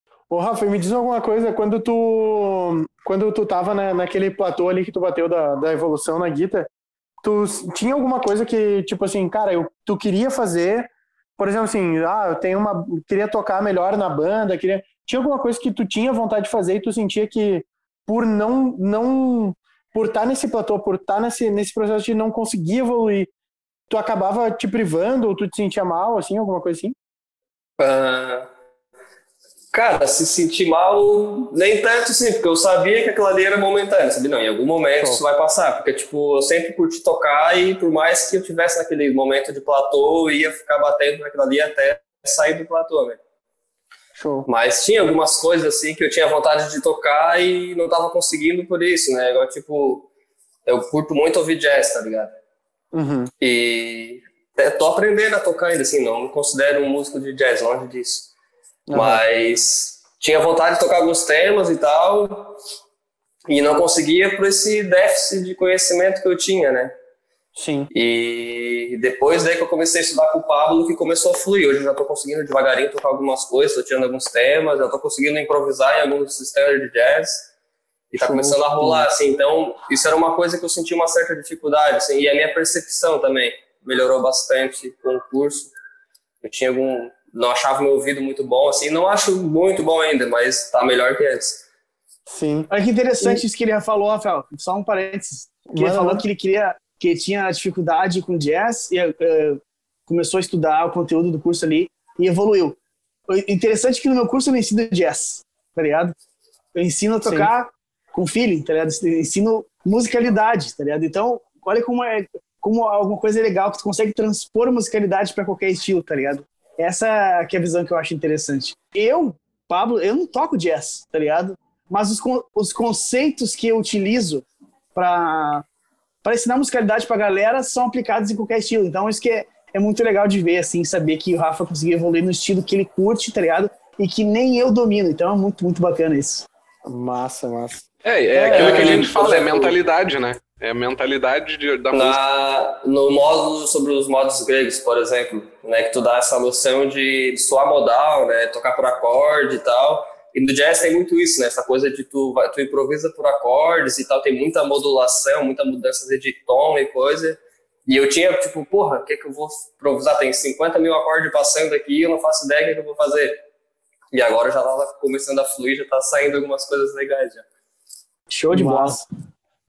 O estudo do FEELING pode ser aplicado em qualquer estilo? | Corte Palhetada | Pablo
Rockeiro, jazzista e compositor - Um bate-papo com o aluno Rafael Silveira | PALHETADA Podcast Ep 07

Receba conteúdos exclusivos e antecipados
Grupo Telegram

 Pablo Klein - Lives Semanais